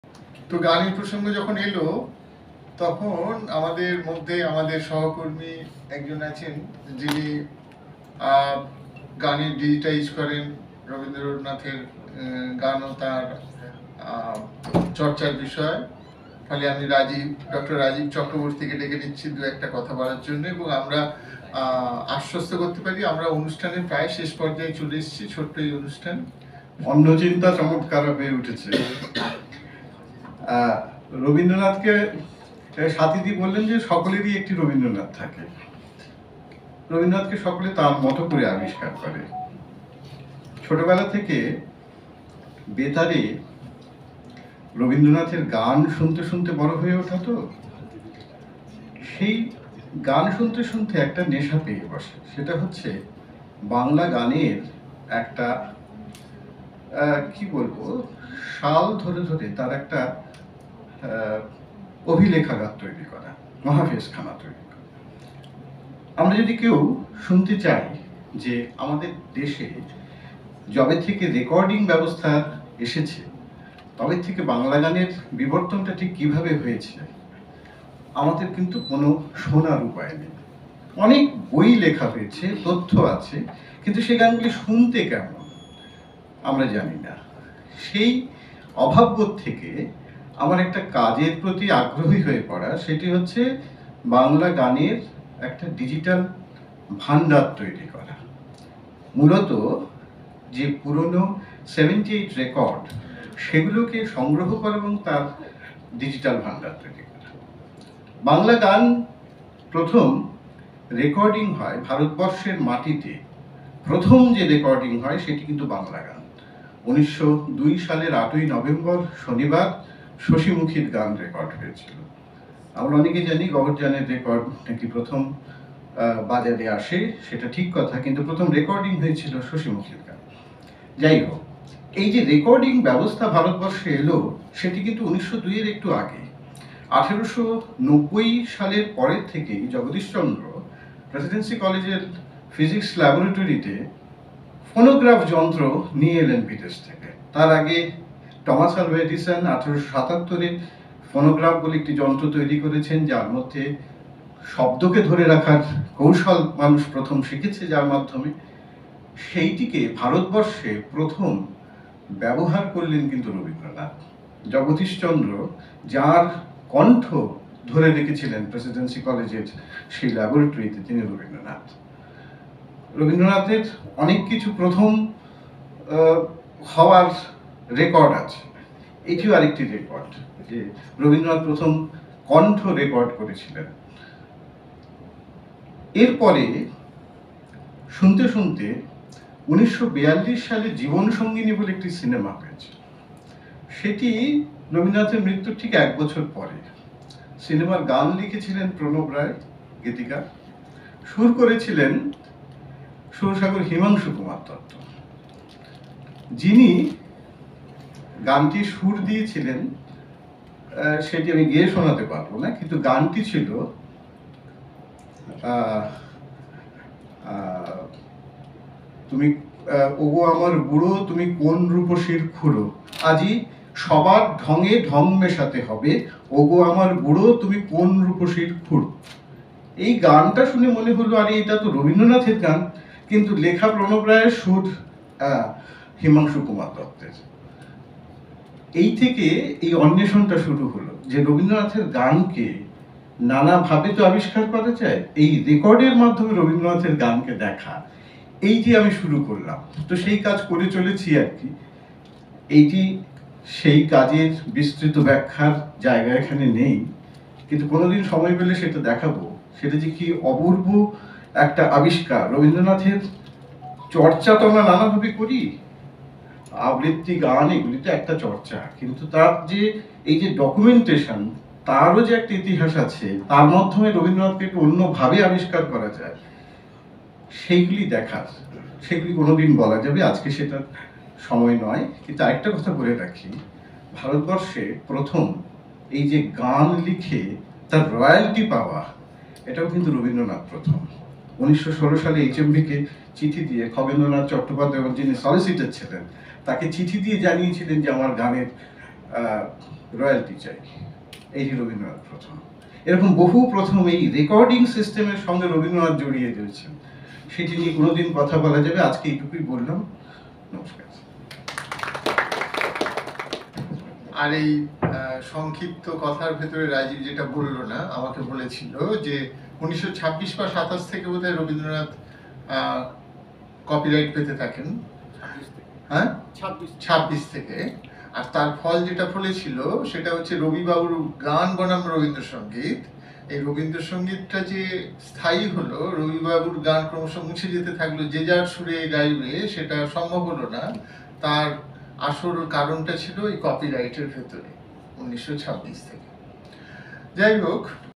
Prof. Rajib Chakraborty talked on Rabindra Jayanti 2024
Institute of Language Studies and Research, Kolkata